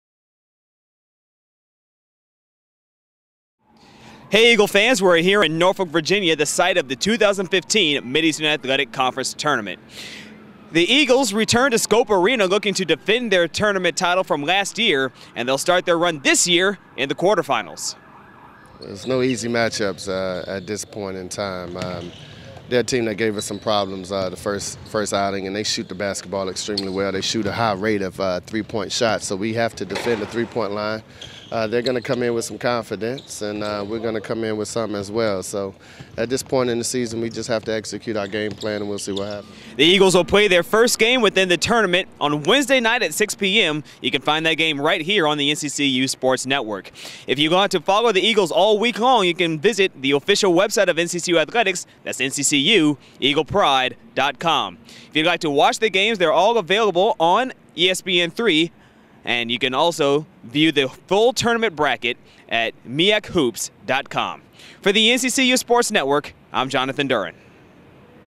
MEAC Tournament Preview
NCCU Head Coach LeVelle Moton gives a preview of the Eagles' first MEAC Tournament opponent in the Eagles of Coppin State.